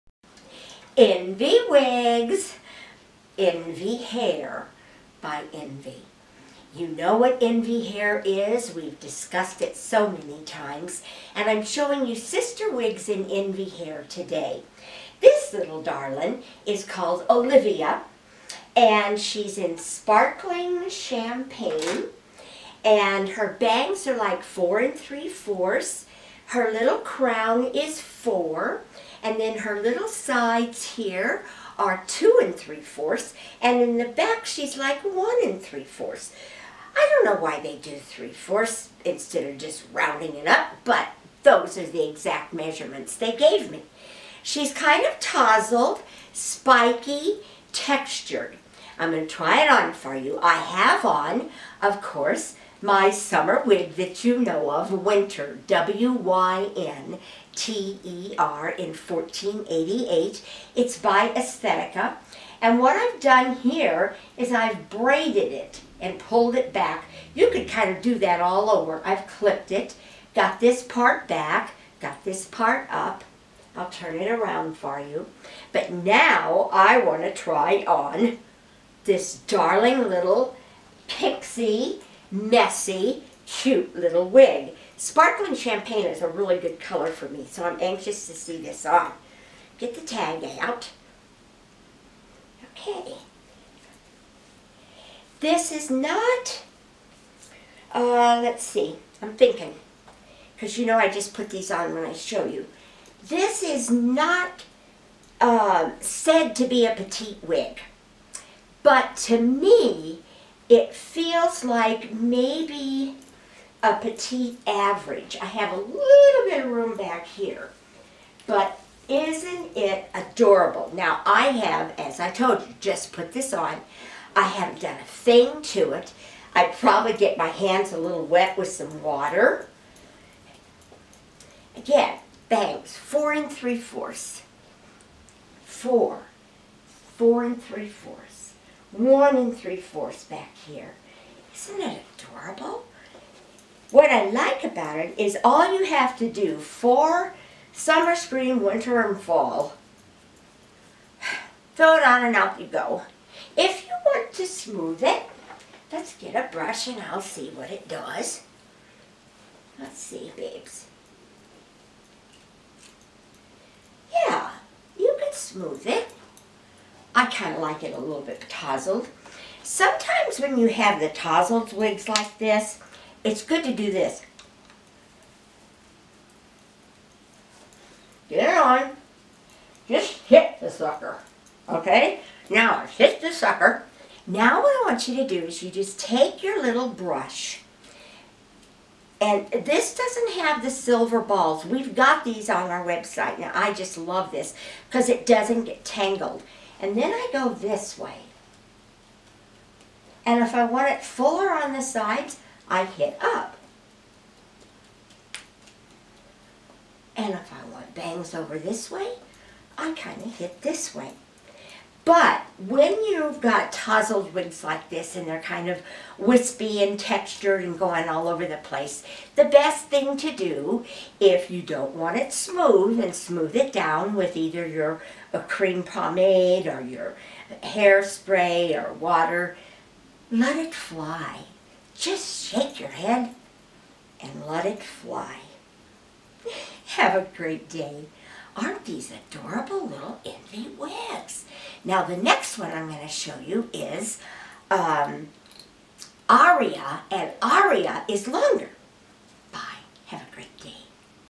Wig Review: Olivia by Envy in Sparkling Champagne Rooted
Patti reviews Olivia by Envy in Sparkling Champagne Rooted. At the beginning of the video, Patti is wearing Wynter by Estetica in RH1488.